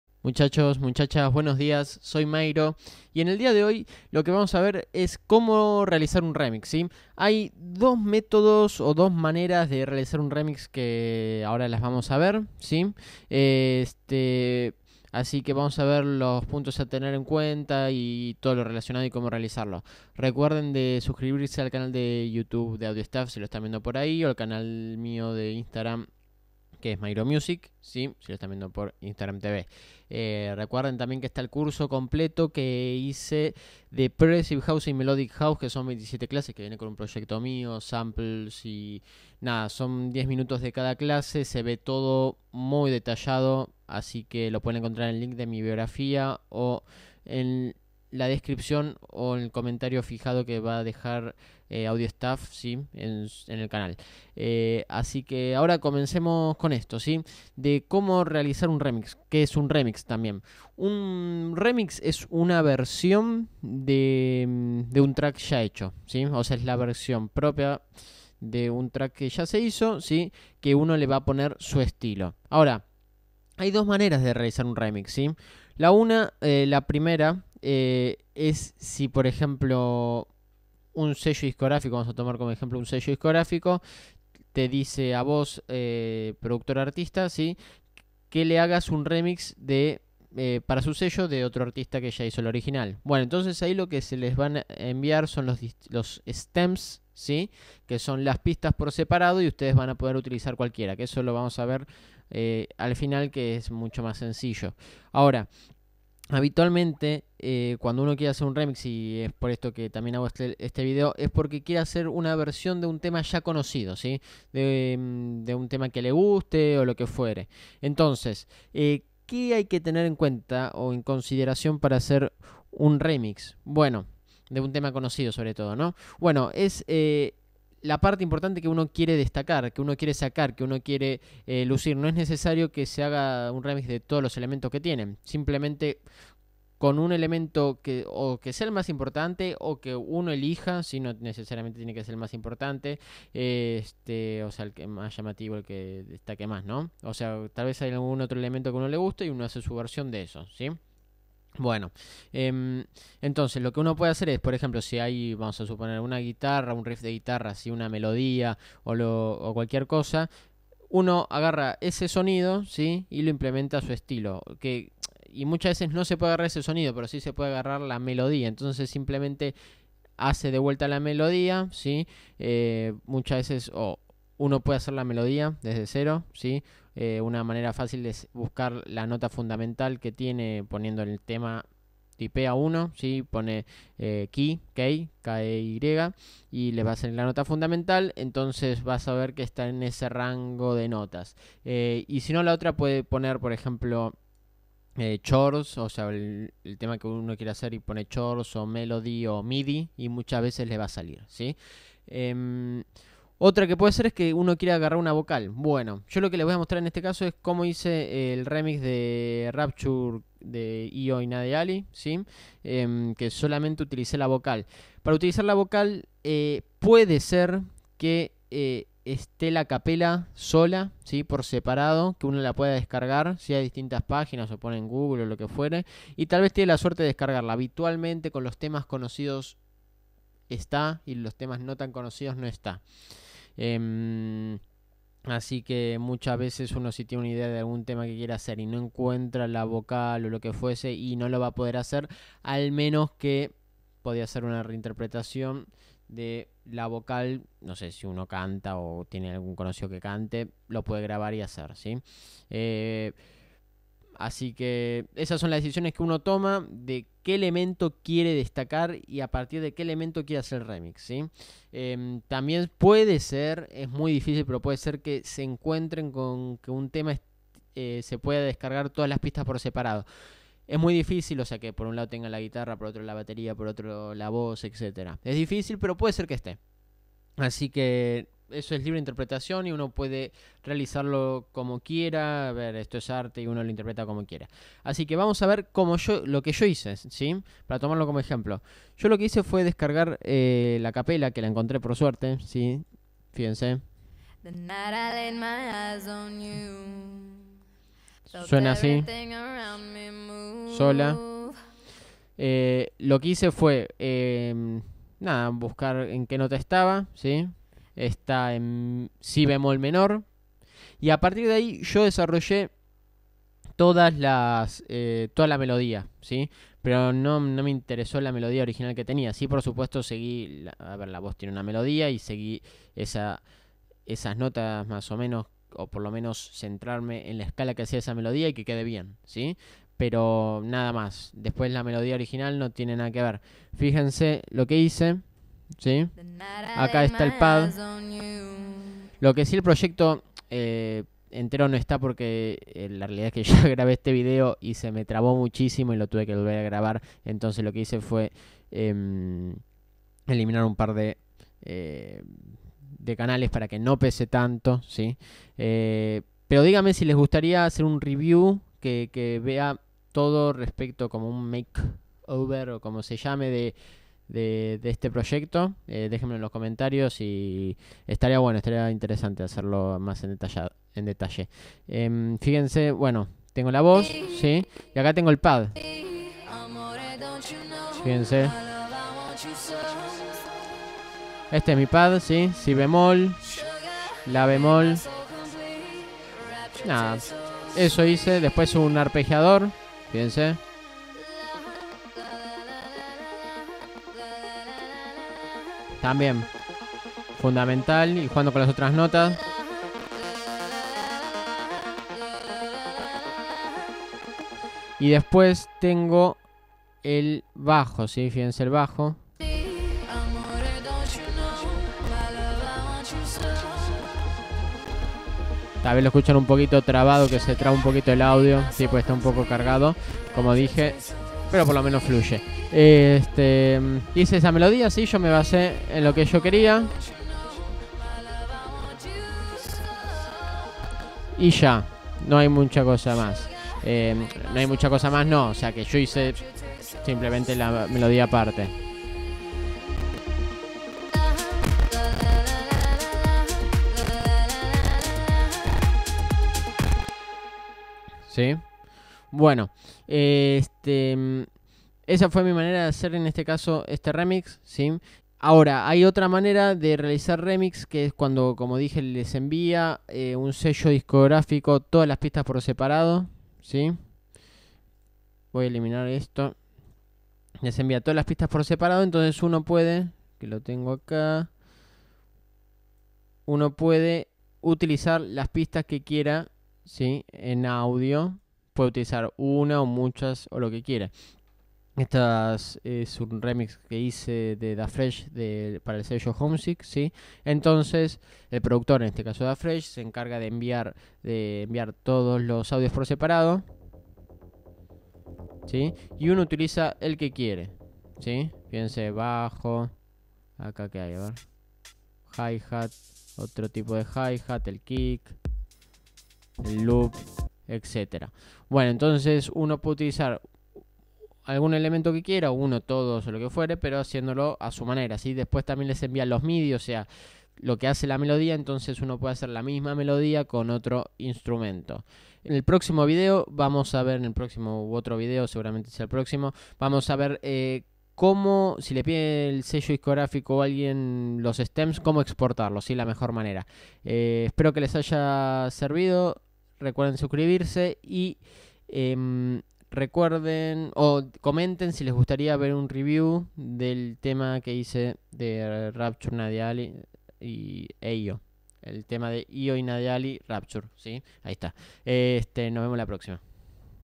Como hacer un remix | MAYRO | AUDIOSTAFF.PRO
En este video, Mayro nos muestra su técnica para hacer un remix de cualquier canción.

¿Te gusto este video?
En nuestra academia, tenemos cursos y abierta la inscripción para que puedas aprender a fondo todo lo que necesitas para mejorar tu música